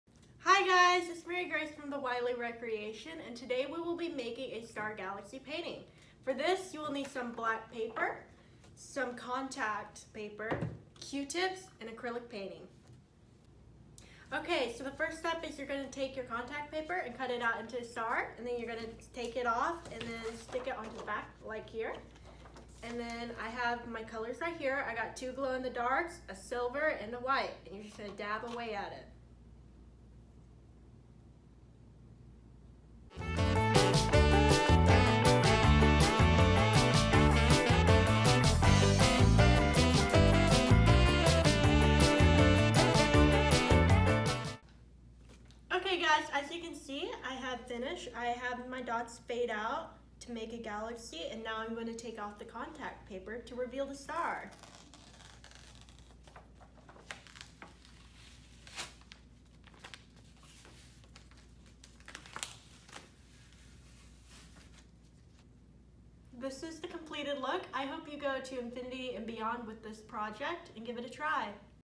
How To - Galaxy Star Painting
Art that is out of this world. Ditch the paint brush and discover how to create a celestial painting using cotton swabs. Follow Mary Grace's step-by-step instructions for this super fun and easy art project.